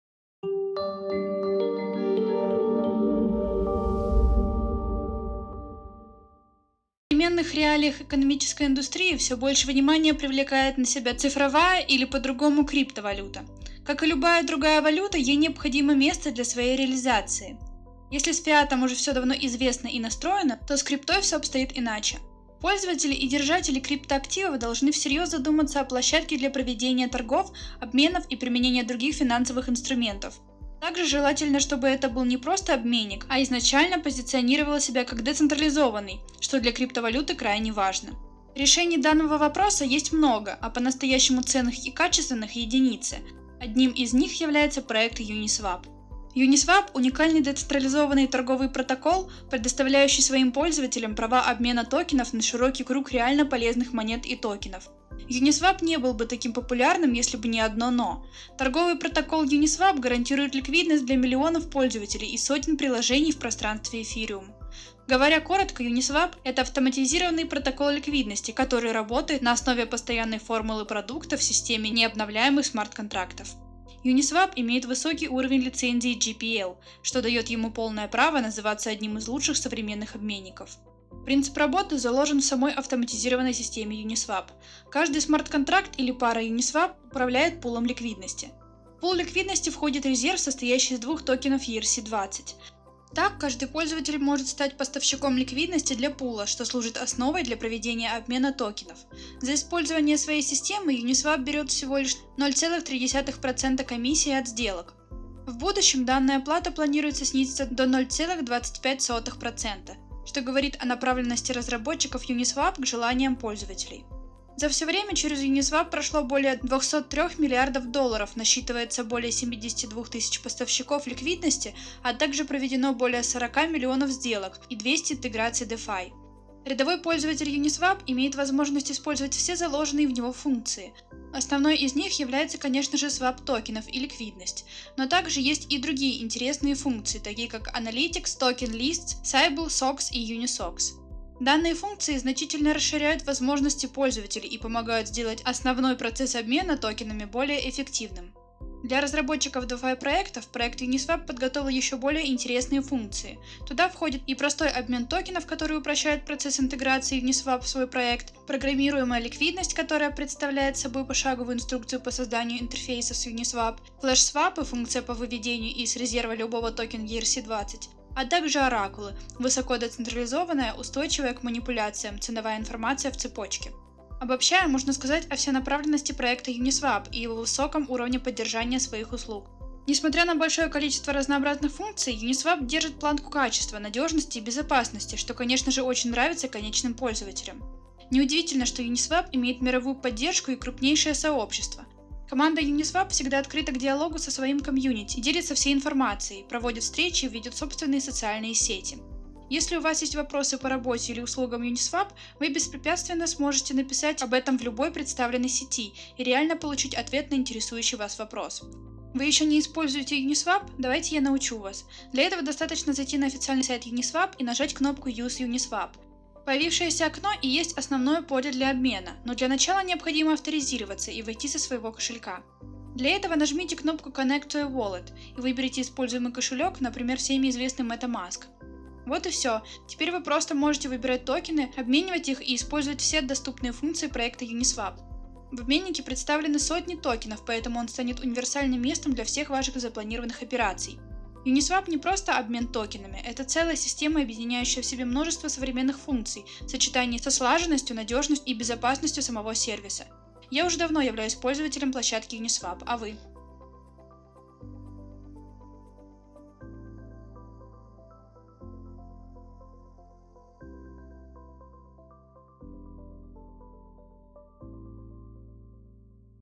🍀 ЧТО ТАКОЕ UNISWAP? // Как Проводить Операции через Metamask?
В современных реалиях экономической индустрии все больше внимание привлекает на себе цифровая или, по-другому, крипто-валюта. Как и любая друга валюта ей необходимо место для своей реализации. Если с Фиатом уже все давно известно и настроено, но с криптой все обстоит иначе. Пользователи и держатели крипто-активов должны всерьез задуматься о площадке для проведение торгов, обменов и применения других финансовых инструментов. Также, желательно чтобы это был не просто обменик, а изначально позиционировал себя, как децентрализованный, что для крипто-валюты крайне важно. Решений данного вопроса есть много, а по-настоящему ценных и качественных – единицы. Один из них является проект Uniswap.

Uniswap – уникальный децентрализованный торговый протоков, предоставляющий своим пользователем права обмена токенов на широкий круг реально полезных монет и токенов. Uniswap не был бы таким популярным, если бы не одно НО. Торговый протокол Uniswap гарантирует ликвидность для миллионов пользователей и сотен приложений в пространстве Ethereum.

Говоря коротко, Uniswap – это автоматизированный протокол ликвидности, который работает на основе постоянной формулы продукта в системе не обновляемых смарт-контрактов. Uniswap имеет высокий уровень лицензии GPL, что дает ему полное права называться одним из лучших современных обмеников.
Принцип работы заложен в самой автоматизированной системе Uniswap. Каждый смарт-контракт или пара Uniswap управляет пулом ликвидности. В пул ликвидности входит резерв, состоящий из двух токенов ERC-20. Так, каждый пользователь может стать поставщиком ликвидности для пула, что служит основой для проведения обмена токенов. За использование своей системы, Uniswap берет всего лишь 0,3% комиссии от сделок. В будущем данная плата планируется снизиться до 0,25%, что говорит о направленности разработчиков Uniswap к желаниям пользователей.

За все время через Uniswap прошло более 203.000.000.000 долларов, насчитывается более 72 тысяч поставщиков ликвидности, а также проведено более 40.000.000 сделок и 200 интеграций DeFi.

Менеджер:  @DmGlyzin